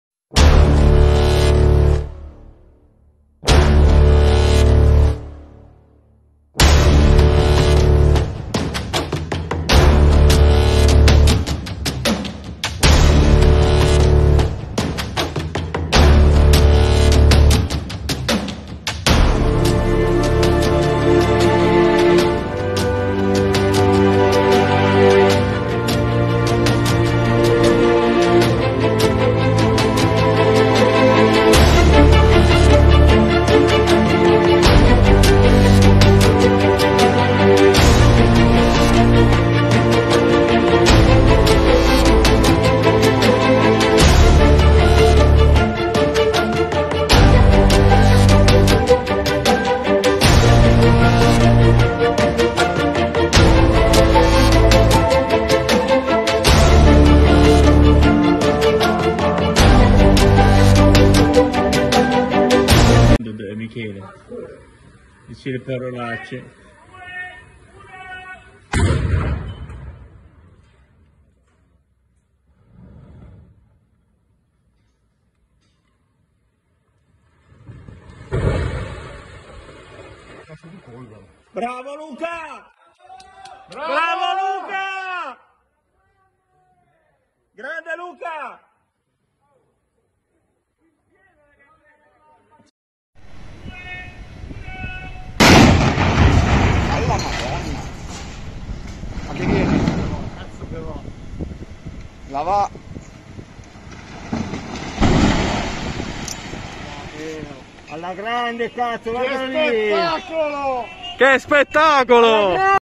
Tecnomine: Demolizione torre piezometrica a Novate Milanese (MI)
Tecnomine demolizione di una torre piezometrica alta 42 m nel comune di Novate Milanese nell'area Ex Ipici, effettuata con soli 1,5 Kg di esplosivo opportunatamente posizionato. Si ringrazia lo studio Civetz Images & Aerial Visions per le riprese con il drone.